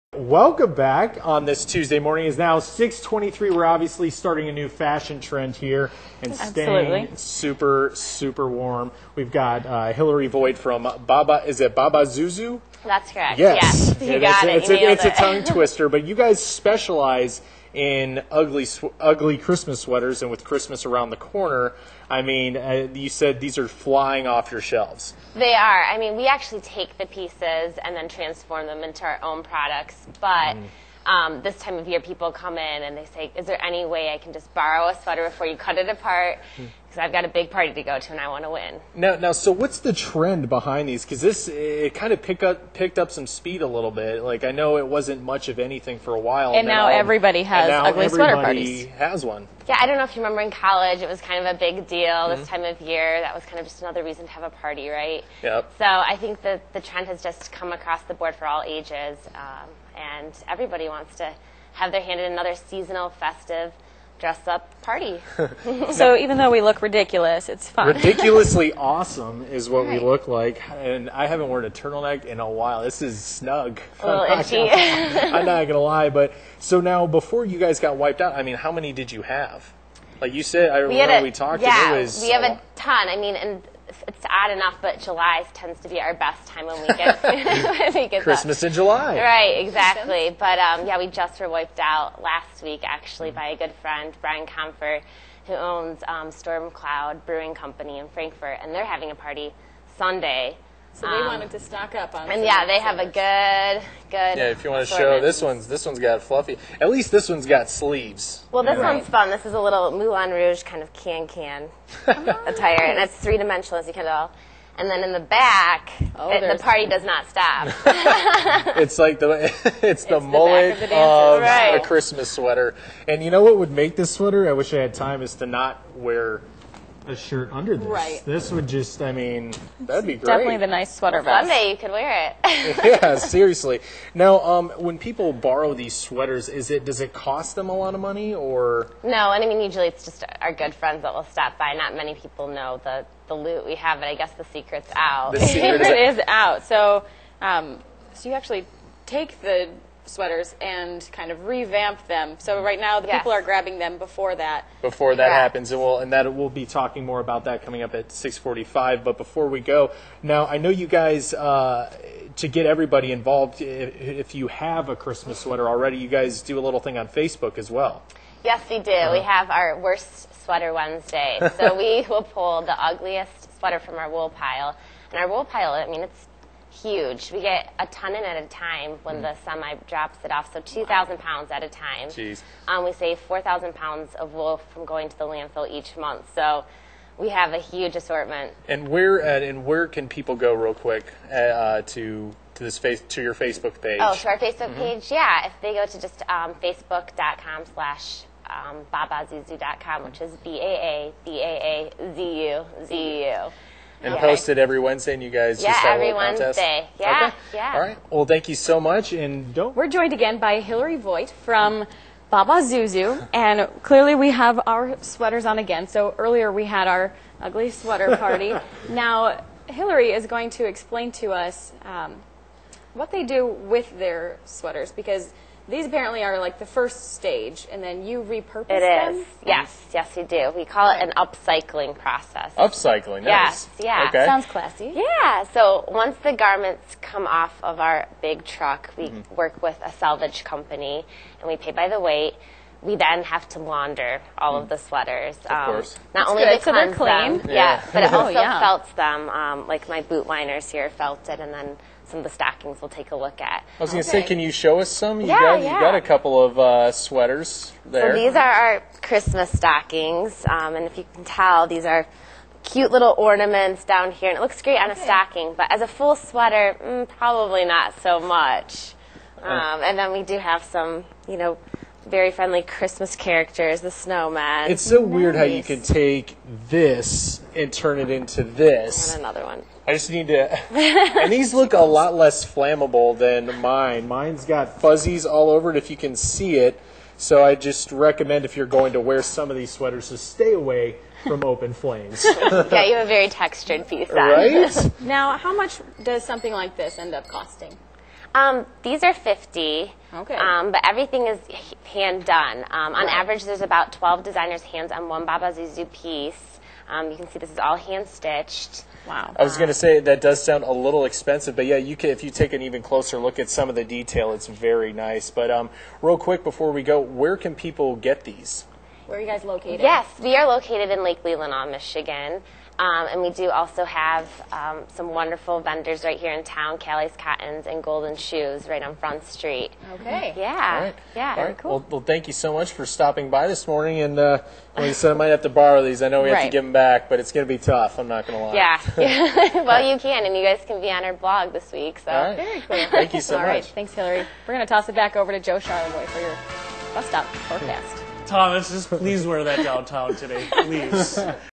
Don we now our ugly Christmas sweaters
Do you or your friends host an ugly Christmas sweater party? A lot of people do. The morning crew tried out some sweaters and learned more about what one northern Michigan clothing company has to offer.